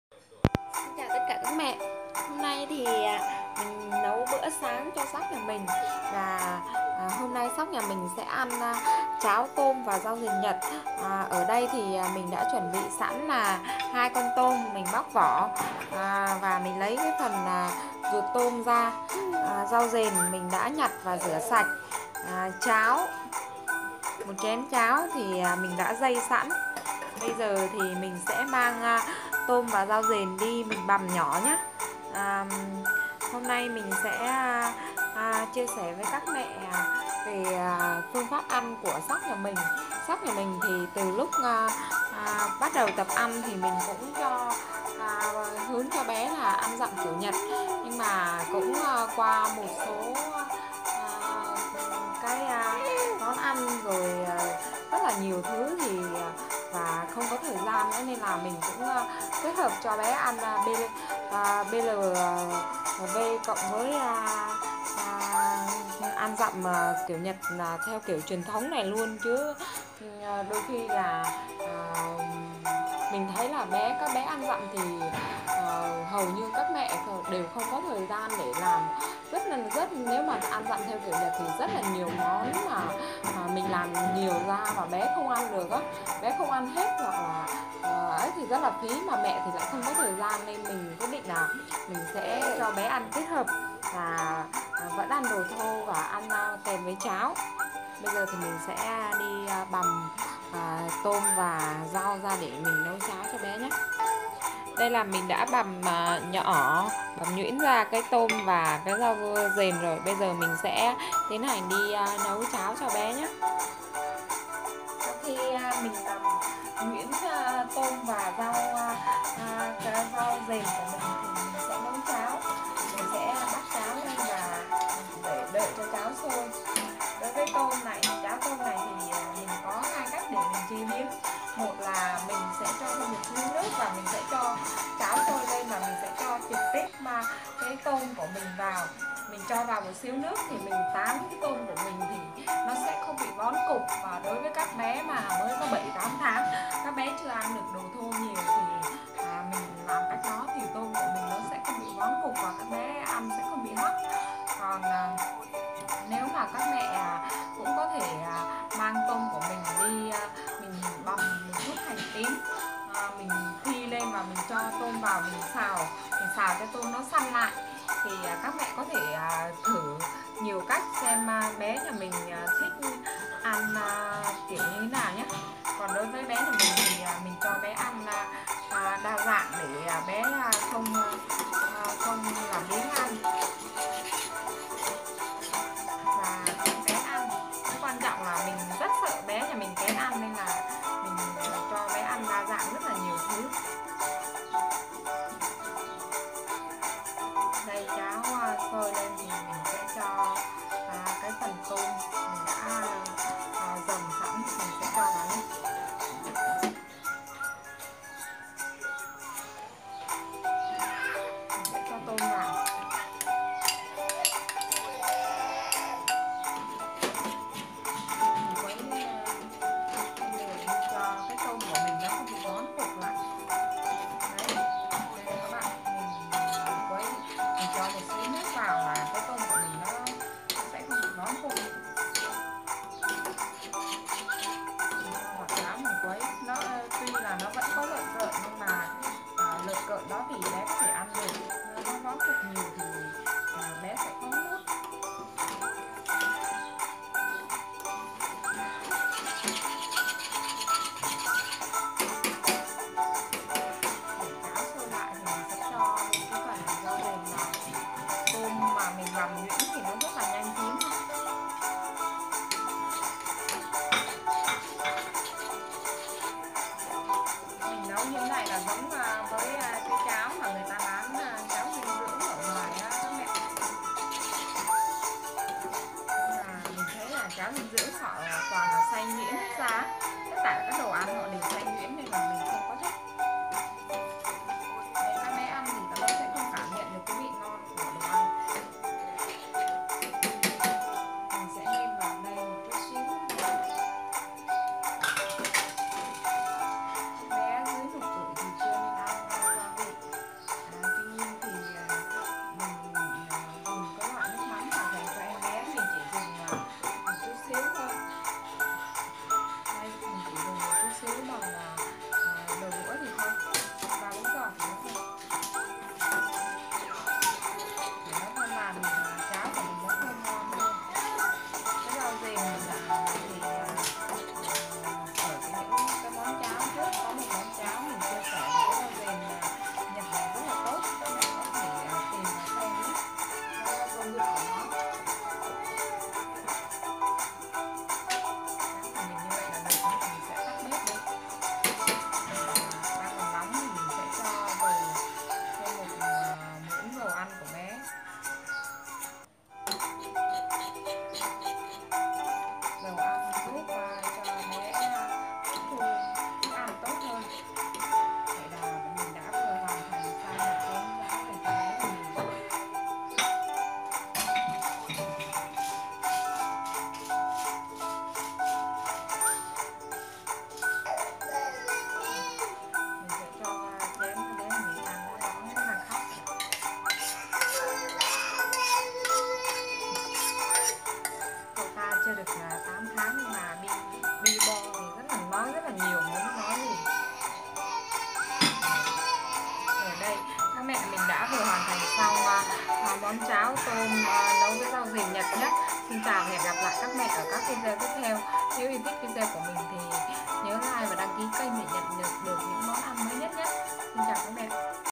Ăn dặm cho bé: CHÁO TÔM RAU DỀN NHẬT -Thanh Tâm Food
Cháo tôm rau dền nhật bổ dưỡng cho các bé. Các mẹ hãy cùng vào bếp để cho bé yêu .